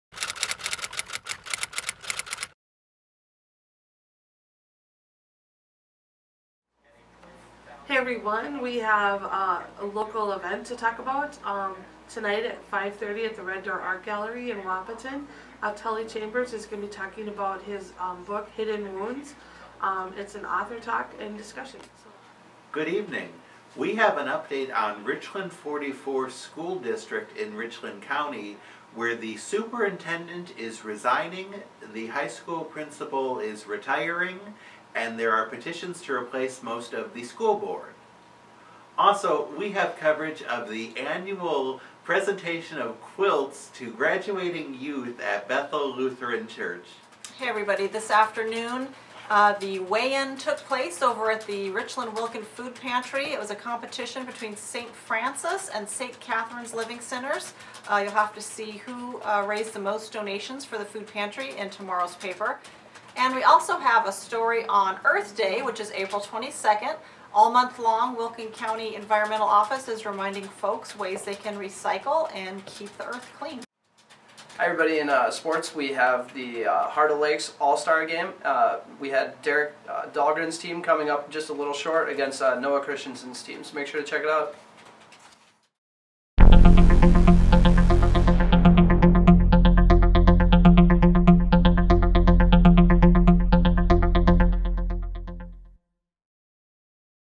Hot Off the Press - preview for Wednesday's paper
A preview of stories in Wednesday's Daily News Media, including the final donation weigh-in at Richland Wilkin Food Pantry and updates to staff changes at Richland 44 school